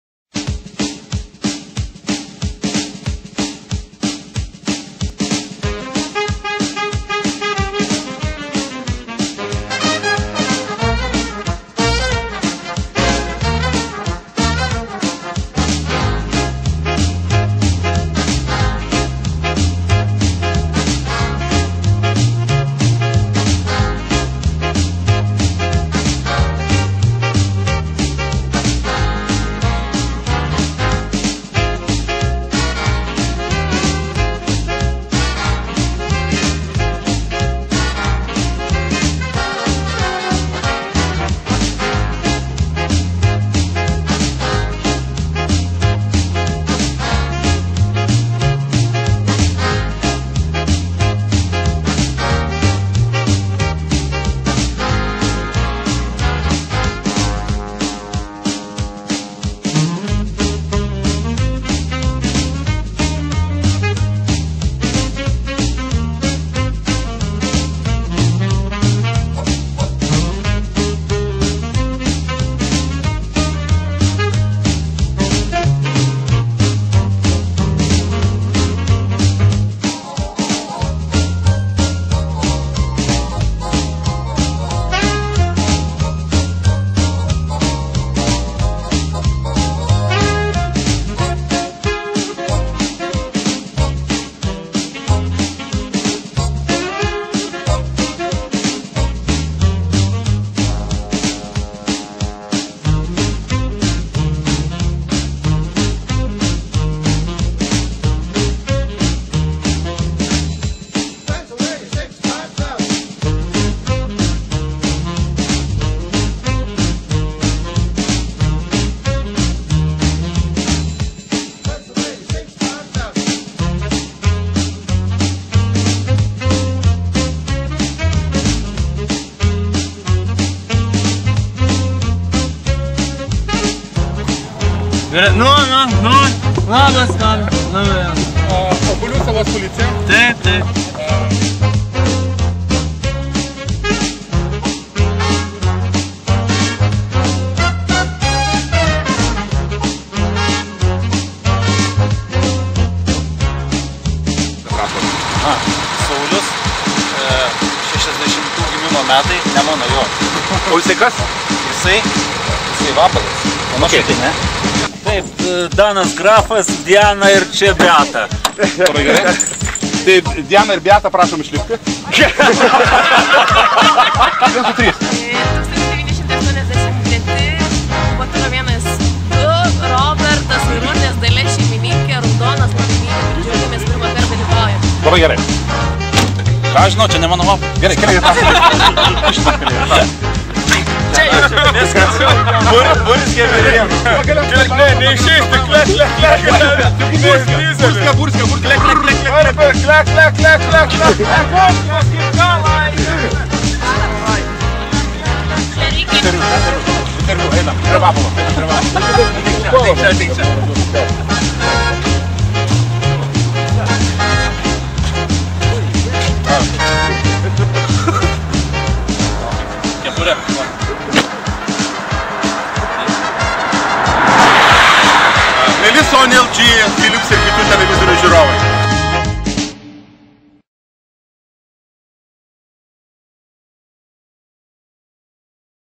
Molėtai 2011 Vabalų sezono uždarymas
Rudeninė vabalų šventė su ekskursija Molėtuose ir pratęsimu Obuolių saloje.